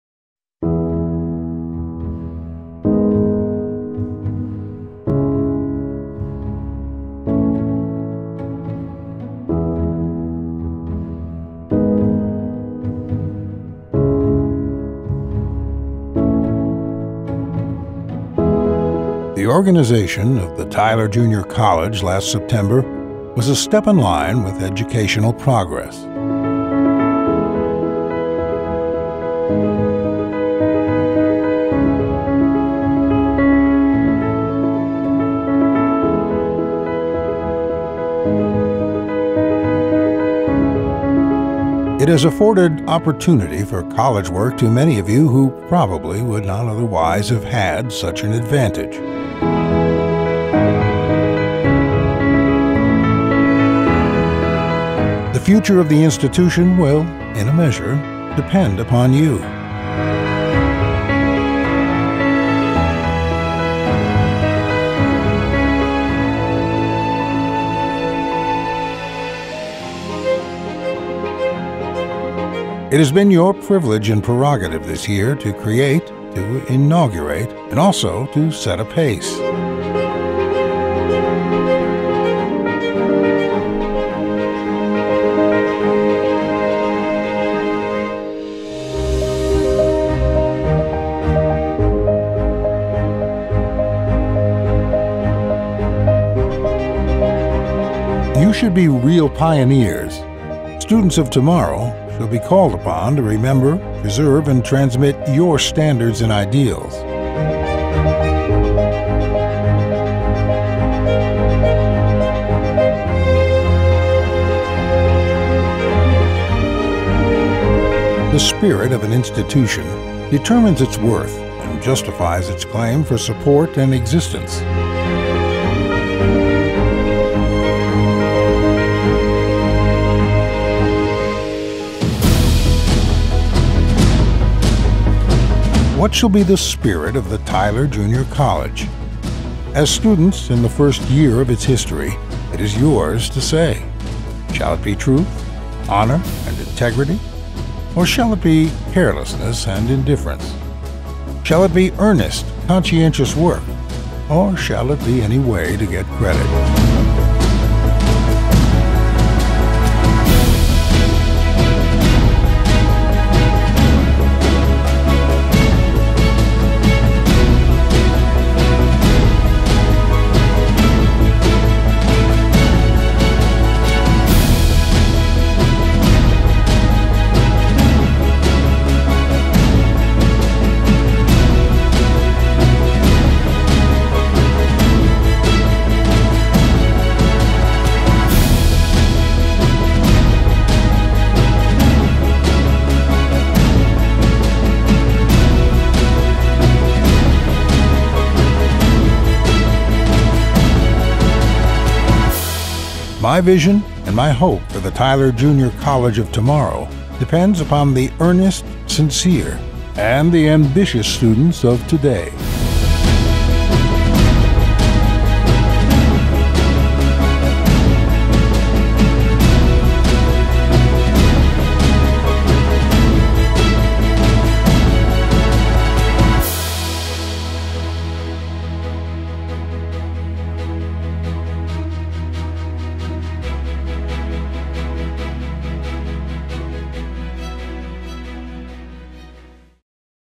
TJC 90th Anniversary Retrospective
For 90 years, TJC has embodied the true sense of the term “community college,” as reflected in its foundational promises to provide students with a quality education, a vibrant student life and opportunities for service to the community.

TJC is one of the largest community colleges in Texas, with 306 full-time faculty members and 248 part-time faculty members, approximately 11,000 students enrolled per semester, and 1,011 May graduates.